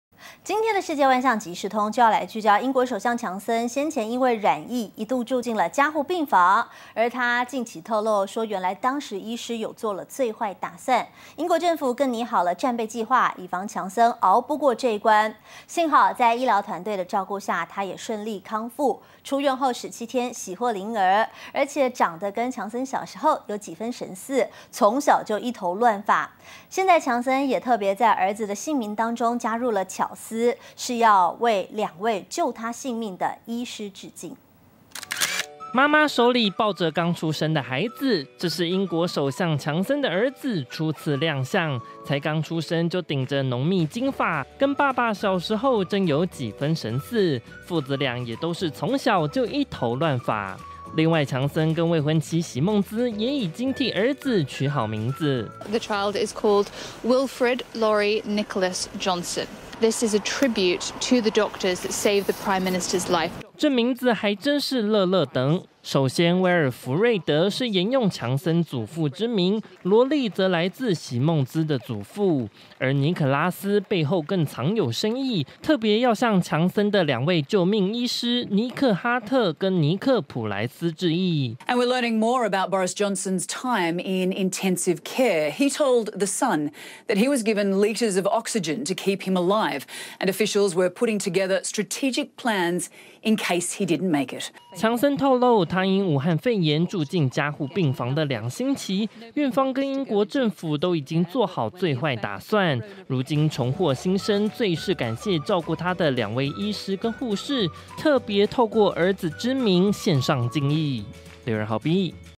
強森兒曝光! 姓名加"尼可拉斯"向醫師致敬
英國首相強森因為感染新冠病毒，一度住進加護病房，他近期透露，當時醫師已經做最壞打算，英國政府也擬好戰備計畫，以防強森熬不過這關。而在醫療團隊照顧下，強森康復出院，更在出院17天後喜獲麟兒，跟強森小時候，有幾分神似，從小就一頭亂髮，強森跟未婚妻，也特地在兒子的姓名中加入「尼可拉斯」，特別向兩位救命醫師致意。